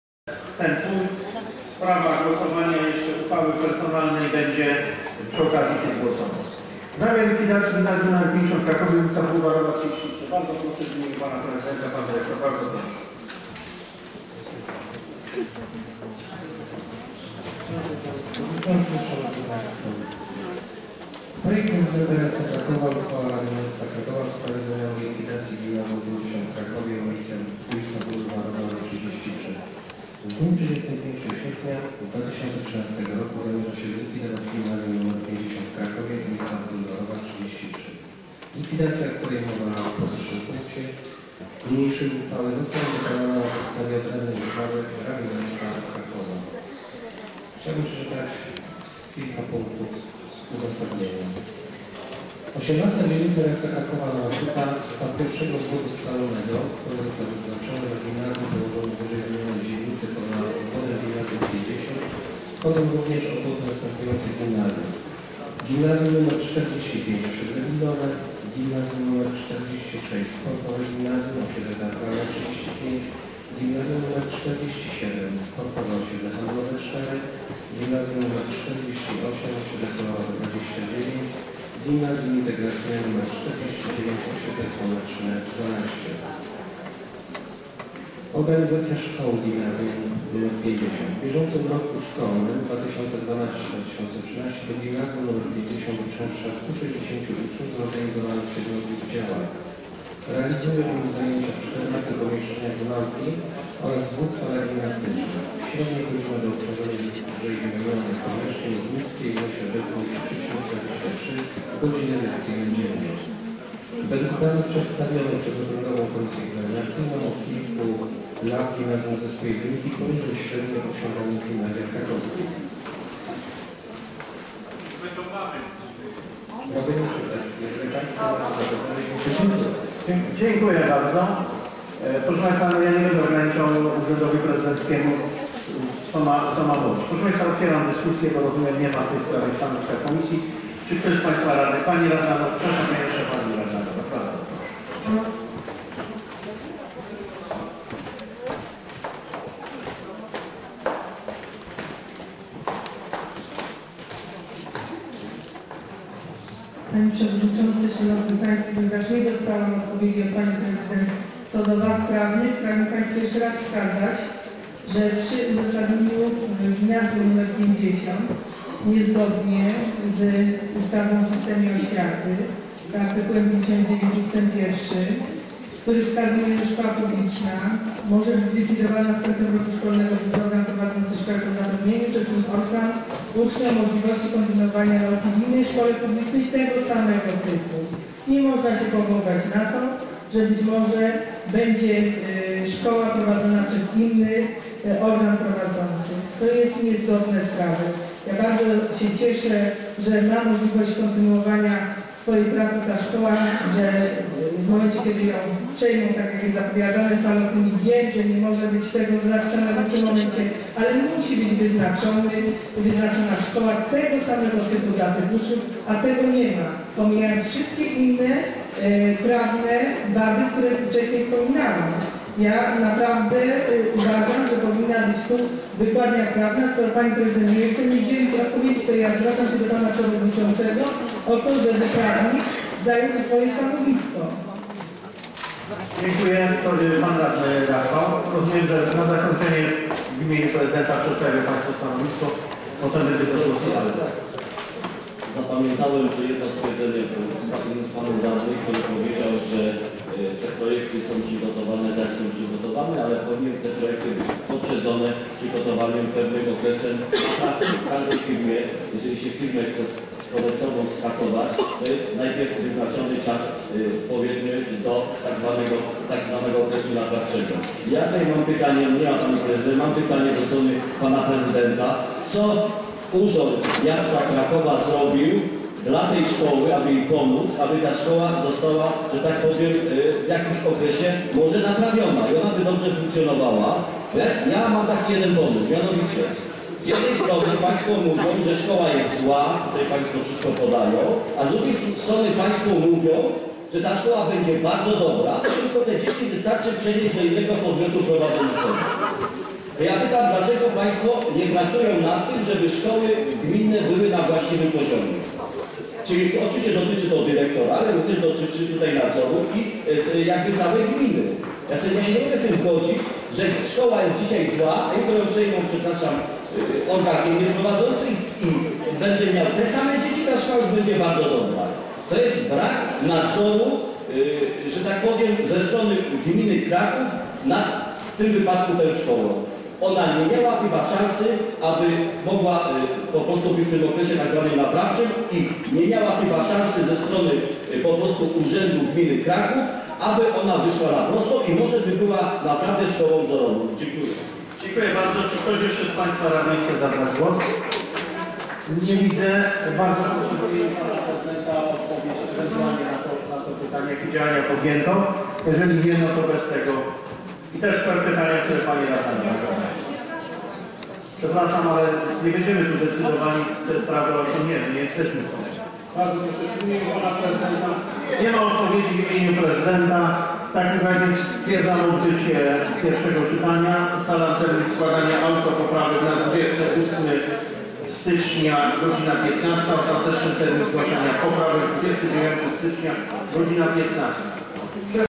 RMK_09-01-2013_27 Zamiar likwidacji Gimnazjum nr 50 w Krakowie, ul. Bulwarowa 33
Obrady LXIV sesji Rady Miasta Krakowa z dn. 9 stycznia 2013.
Zamiar likwidacji Gimnazjum nr 50 w Krakowie, ul. Bulwarowa 33 - I czytanie